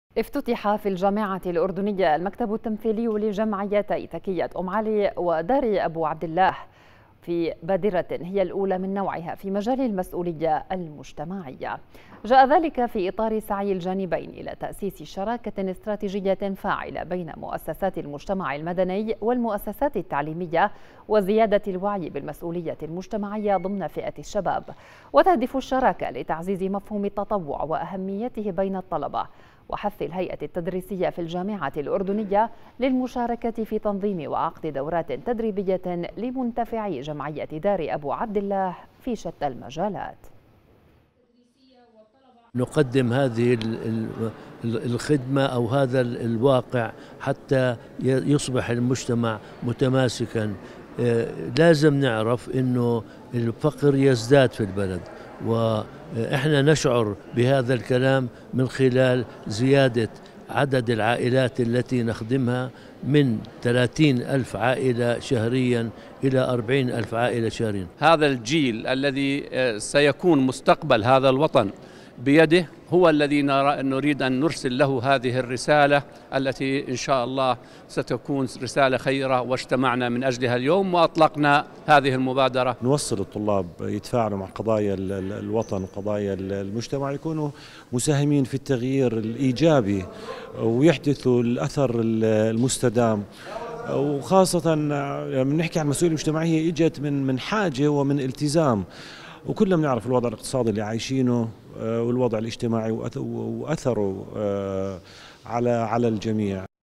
مكتب ارتباط في حرم الجامعة الأردنية لتكية أم علي ودار أبو عبدالله (27-2-2019)
افتُتِحَ في الجامعةِ الأردنية المكتبُ التمثيليُّ لجمعيتيّ تكية أم علي و دارِ أبو عبدالله، في بادرةٍ هي الأولى من نوعِها في مجالِ المسؤوليةِ المجتمعية.